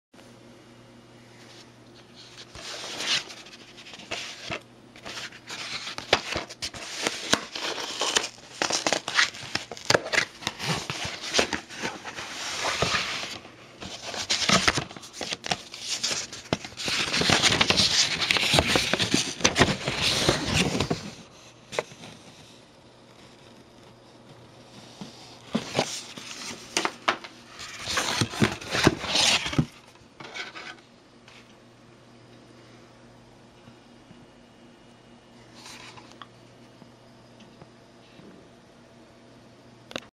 Day 6 TheDazz gets a Large packet through his letterbox 18th January 2012
the postman sticks something through my letterbox again